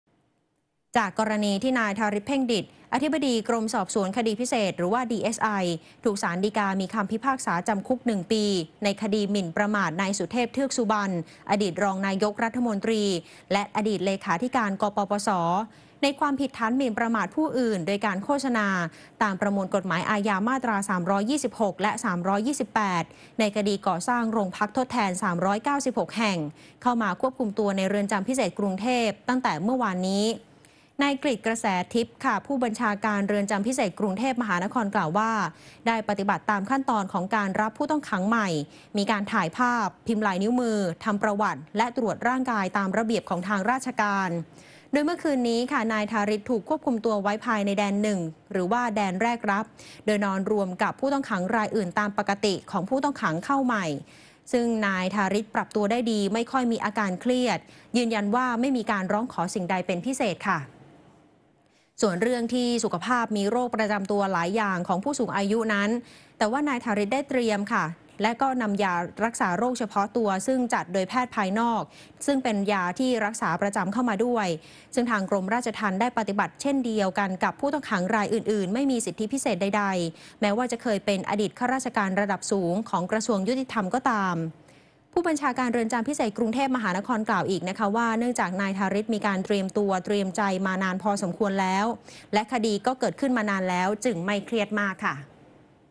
คืนแรกในคุก! ธาริต ไม่เครียด เตรียมตัวดี พกยา ไปด้วย | 15 ธ.ค. 61 | ติดข่าว
ผู้บัญชาการเรือนจำพิเศษกรุงเทพมหานคร เผย "ธาริต"นอนคุกปรับตัวได้ ไม่เครียด ยืนยันว่าไม่มีการร้องขอสิ่งใดเป็นพิเศษ